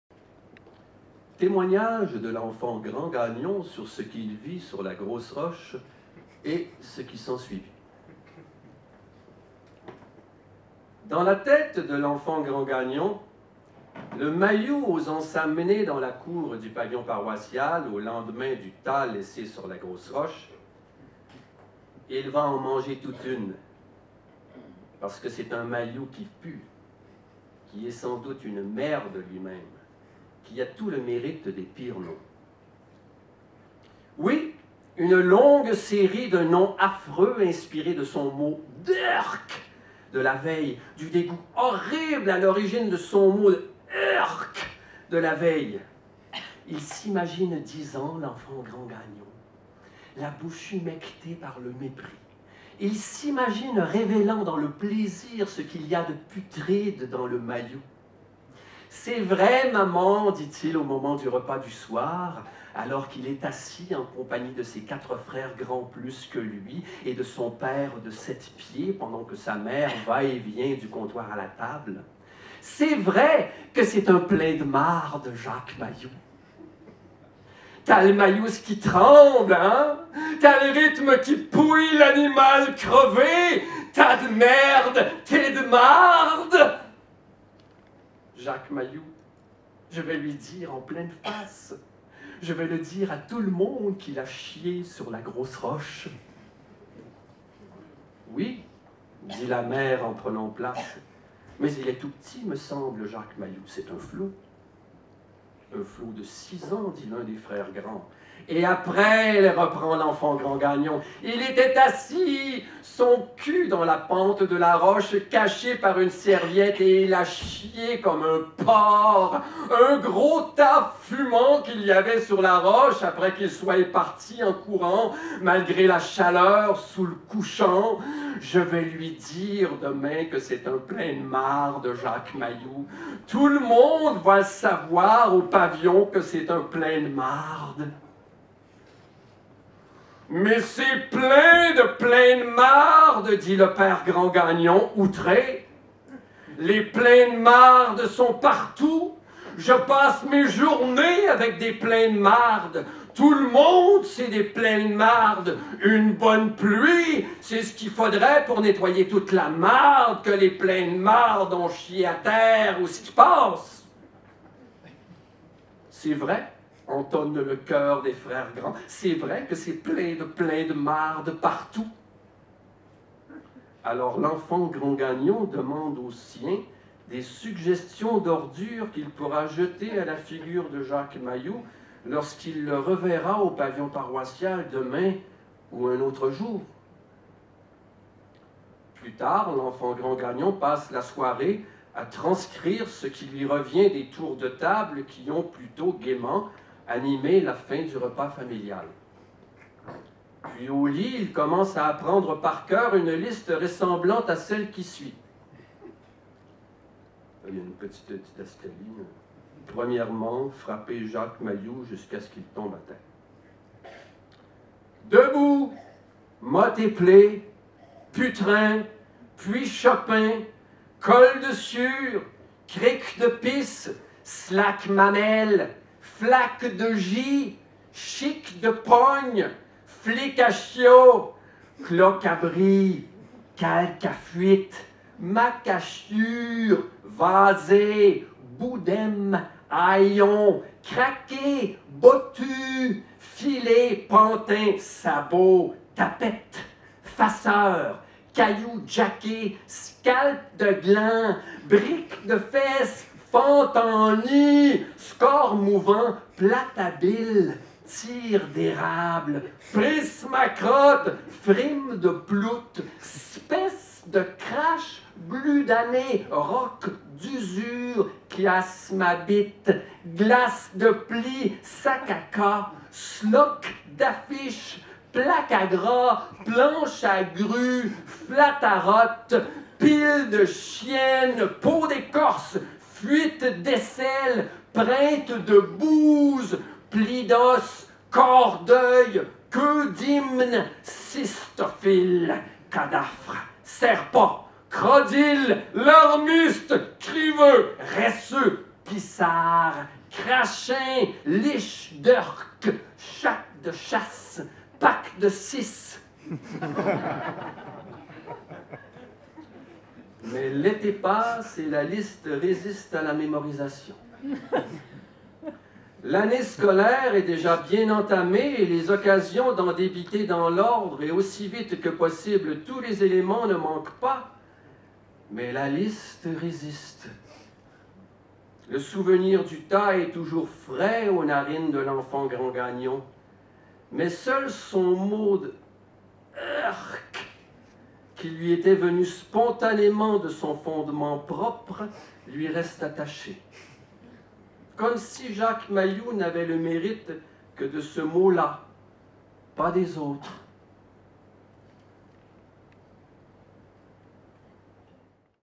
Hervé Bouchard 28/06/19 Paris, Pan Piper
production Le Monte-en-l'air, Murailles Music , le Pan Piper Paris et Paris Sonic.

Le Québécois Hervé Bouchard dérègle la syntaxe et bat le fer du vocabulaire pour révéler l’enfance de vivre à la façon d’un grand théâtre sans bords, sauvage, cruel et drôle où tout s’invente à chaque instant à la barbe des morts, à la barbe de mourir.